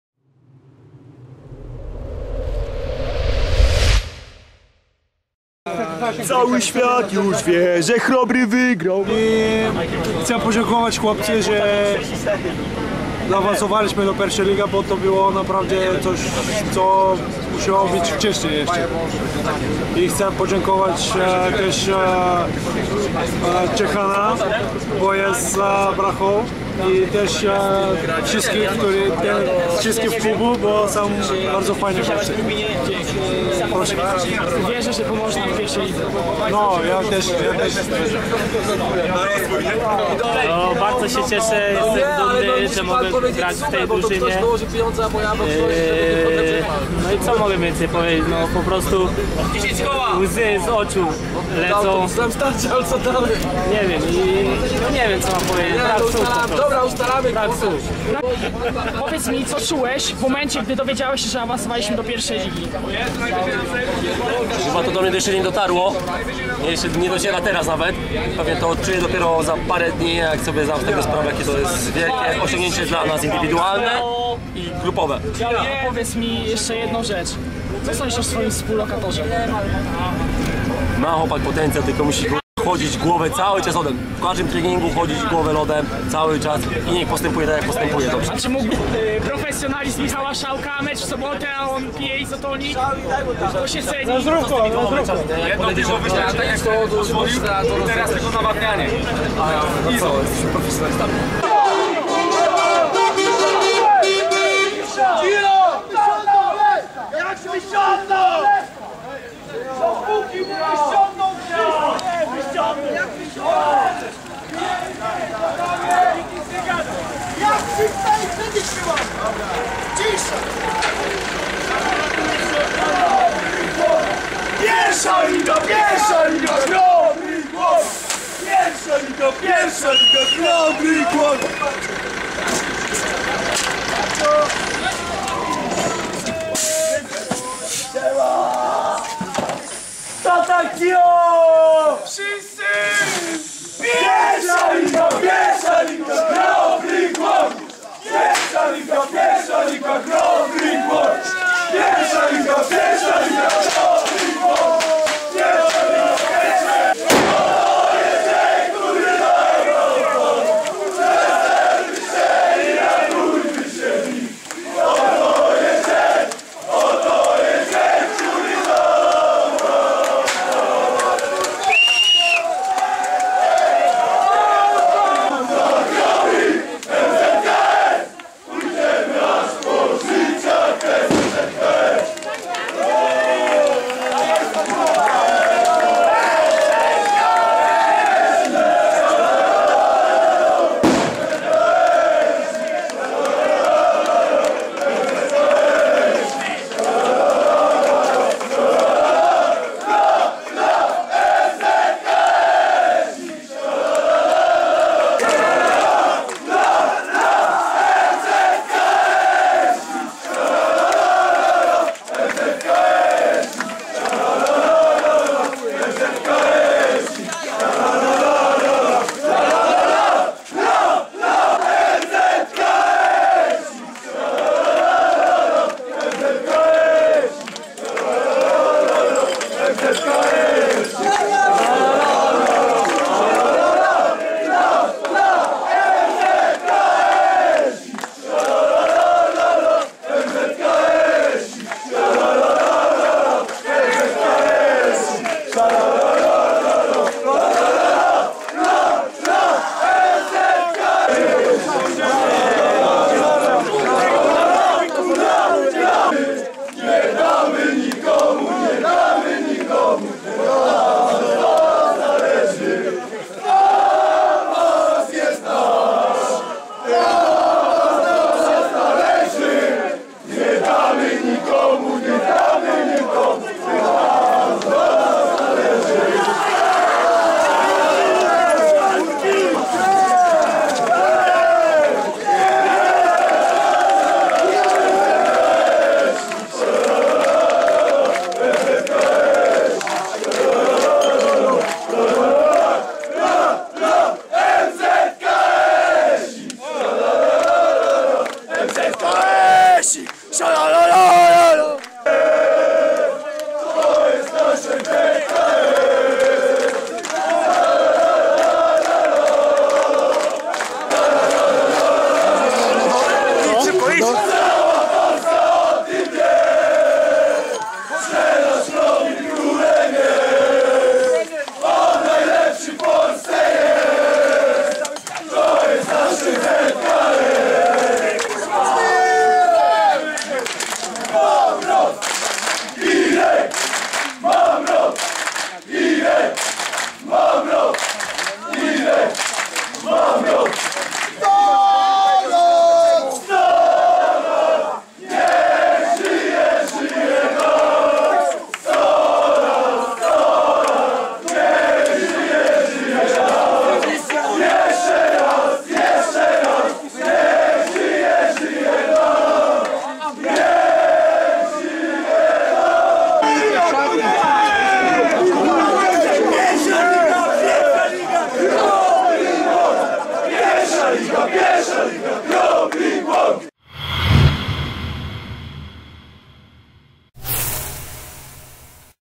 Daniel Ciechański z kamerą w autokarze i przywitanie z kibicami
Na wracających do Głogowa piłkarzy Chrobrego Głogów czekała niespodzianka ze strony kibiców. W nocy ok. 100 osób spontanicznie świętowało awans klubu na zaplecze Ekstraklasy. Ale zanim to nastąpiło miała miejsce inna spontaniczna sytuacja, klubową kamerę wziął w swoje ręce... Daniel Ciechański i przepytywał kolegów z zespołu.